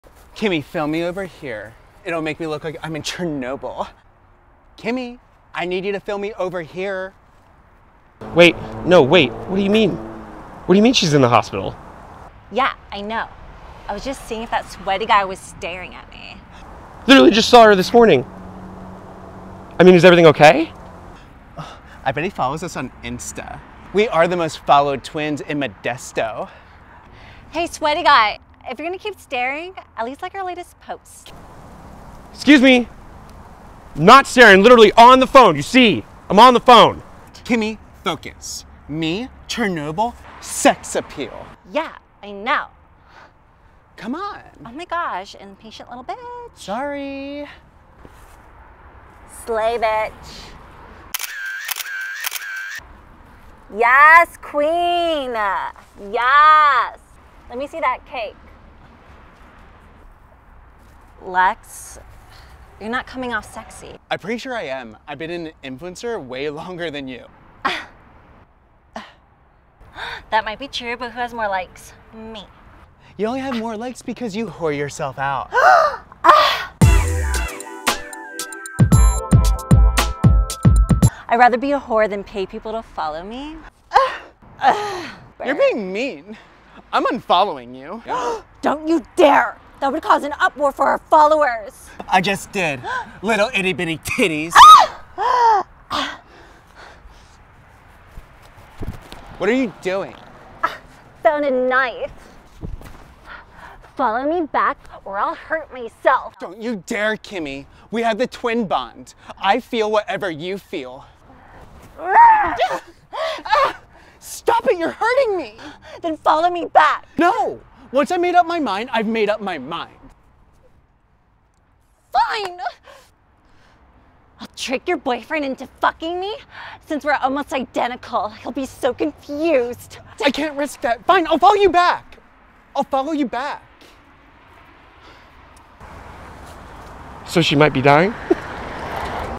Most Famous Twins on Instagram
Hi Youtube sluts it's Lex and Kimmy the most followed twins of Modesto. After watching the trailer of HBO's Chernobyl we thought we would recreate the travesty setting; however, my twin brother Lex decided to unfollow me and that hurt me to my very core. I mean if you unfollow someone on Instagram...it's like you unfollow them from your life. like forever. So I had to take things to the next level, so Lex would know the pain he caused me, I can't be socially humiliated like that. See what happens when desperation kicks in. Also who do you think is the hottest twin? Plus a sweaty guy tries to ruin the whole photoshoot mood, see how we power through and still take fabulous shots.

Cast:
Kimmy: Dennice Cisneros
Lex: Nicholas Creighton
Sweaty Guy: Nidal Bukhari

Sound: Nidal Bukhari
Editing: Jordan Wilson